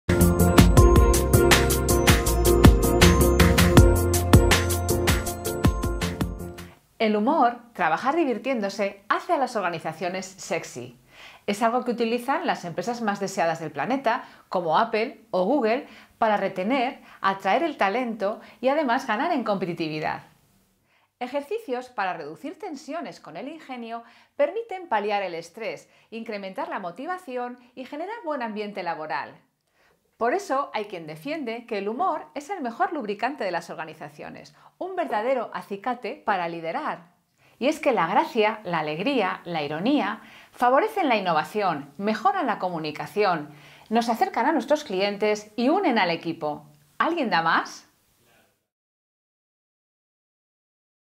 Luisa Alcalde: Lubrícate con humor
El humor, trabajar divirtiéndote hace a las organizaciones “sexy”. Es algo que utilizan las empresas más deseadas del planeta como Apple o Google para retener y atraer talento e incrementar su competitividad.